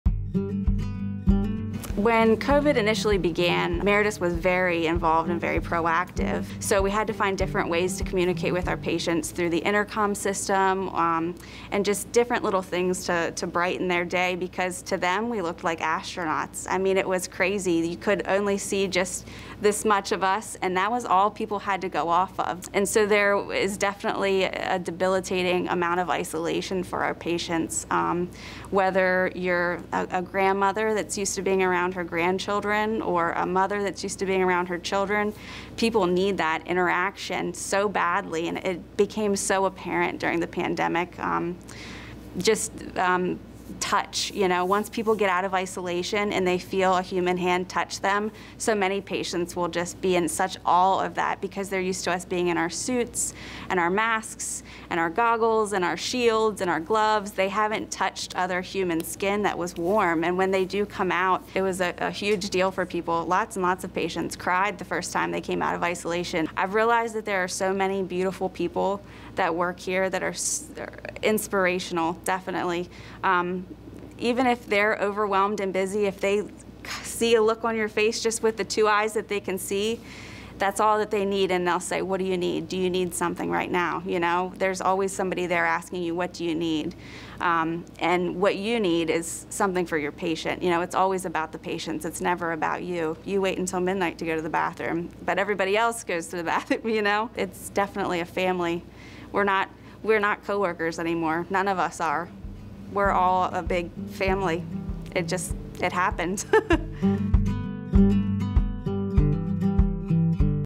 Pandemic Reflections - We looked like astronauts.
COVID-19 patients in isolation struggled emotionally and Meritus team members found meaningful ways to connect through their layers of PPE.